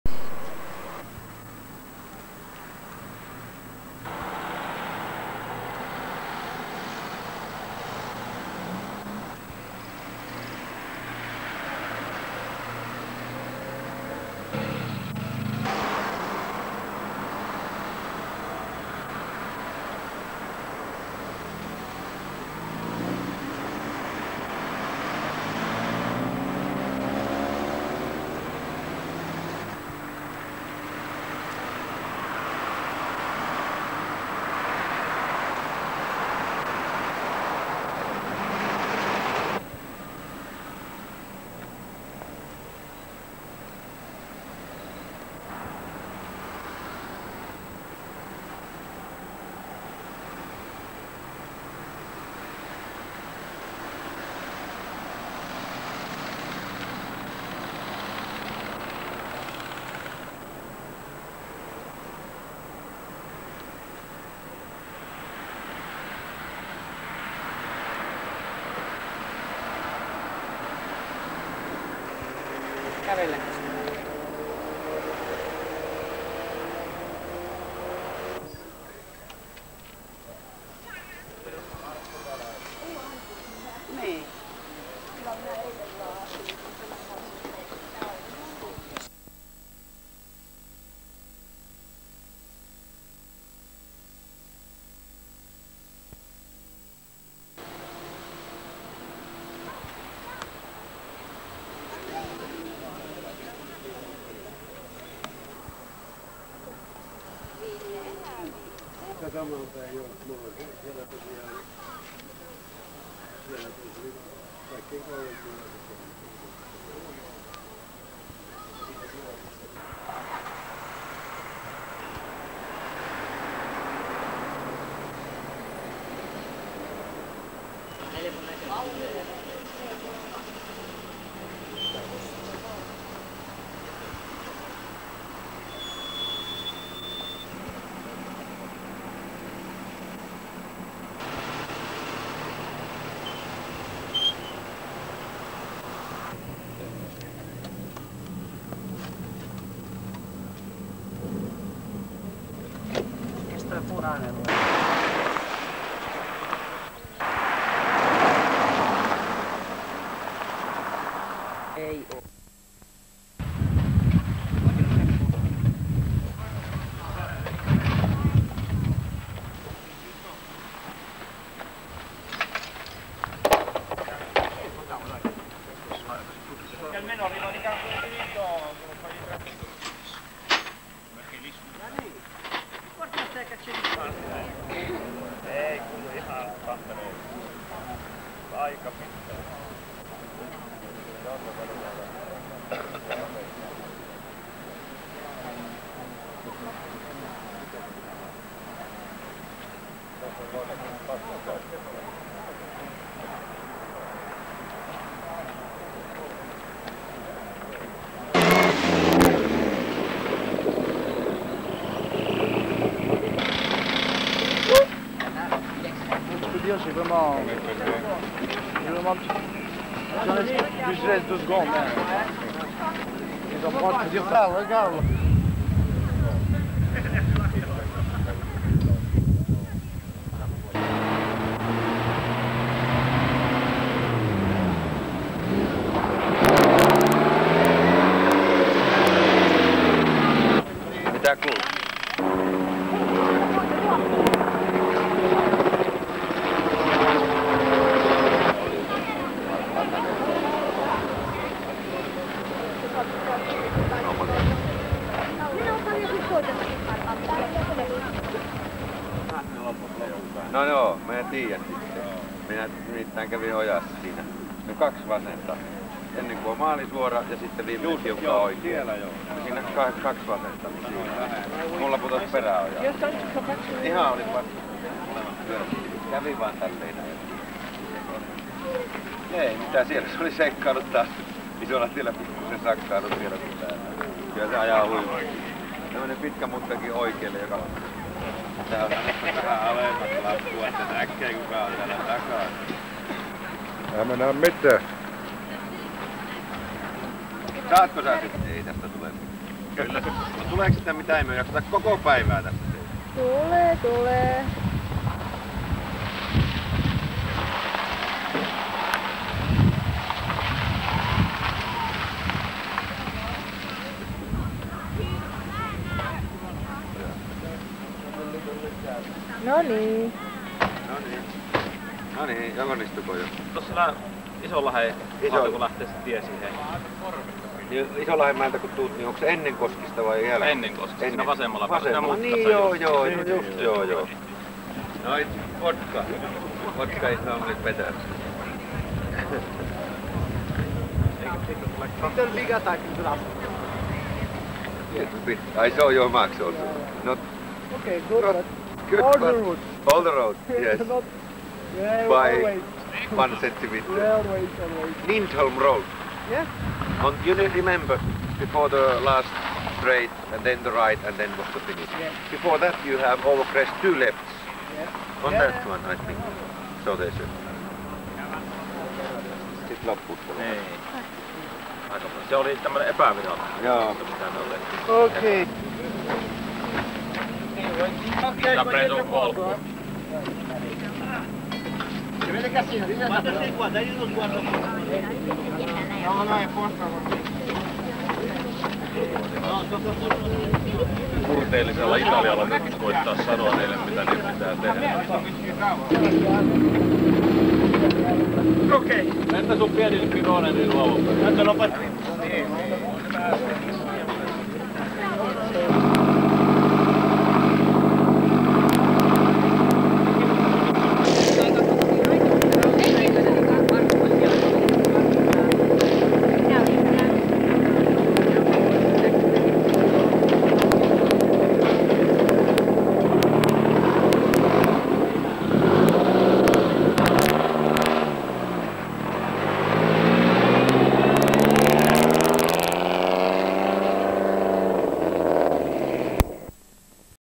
1000 Lakes Rally 1992 (road side service Juha Kankkunen & Juha Piironen, Didier Auriol)
Begin of the video includes short clip from Jämsä, on front of iconic Pub Pikis and small rally exhibition before the Rally at Kesoil gas station. After that there is clip from Lancia Martini road side service base located Puharila school, after SS 20 Ouninpohja before SS 21 Västilä.